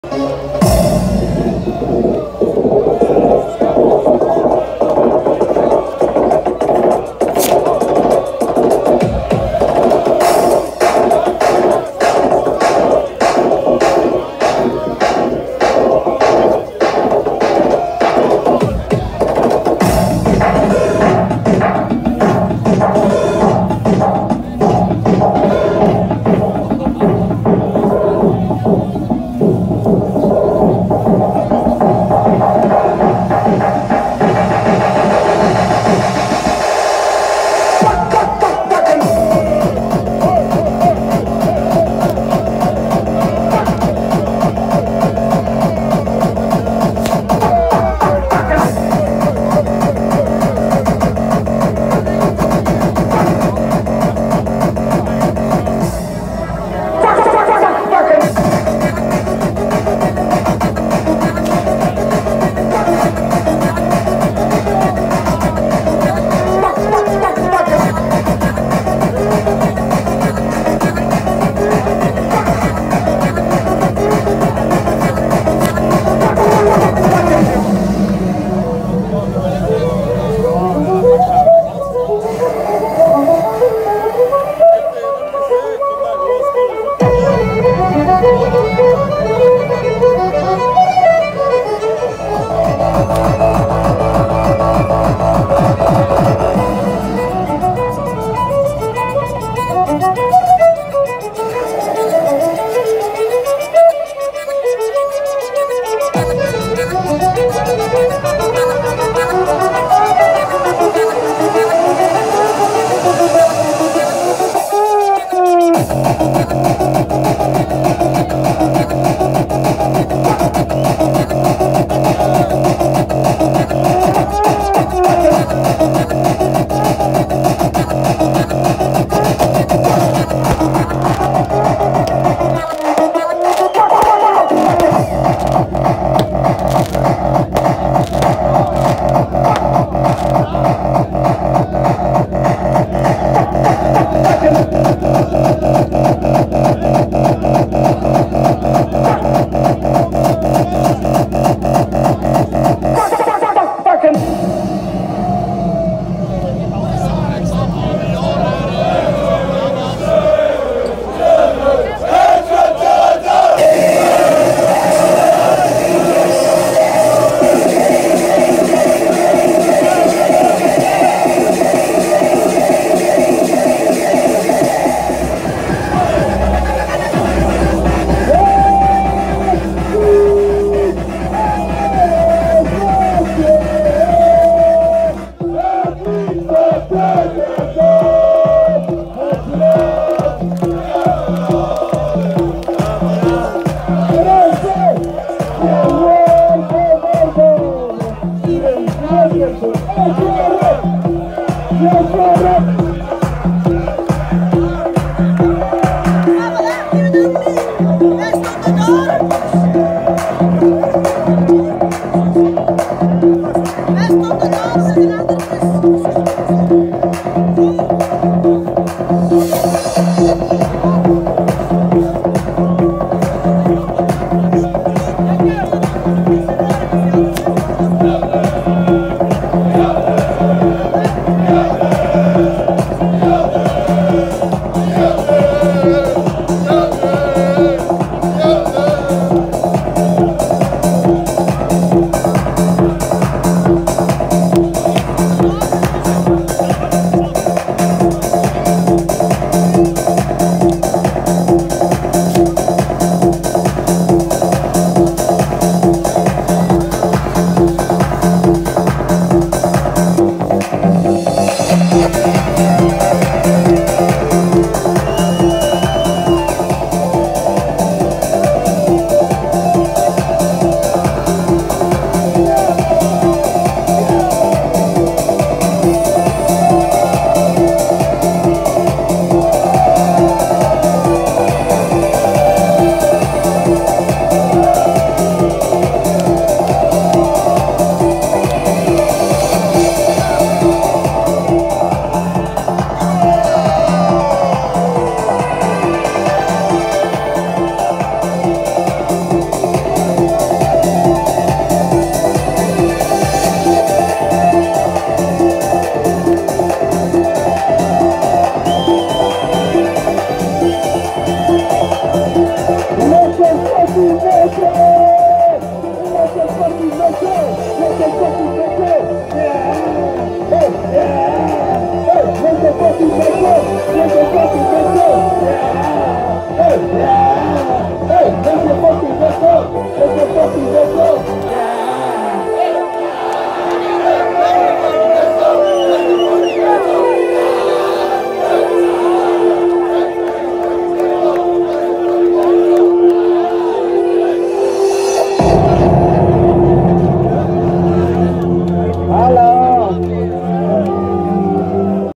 19/05/2019 Sfeer RAFC fans- de parking is van ons
Als de politie ons verplicht laat stoppen op een parking dan maken wij daar het nieuwe Tomorrowland van!!!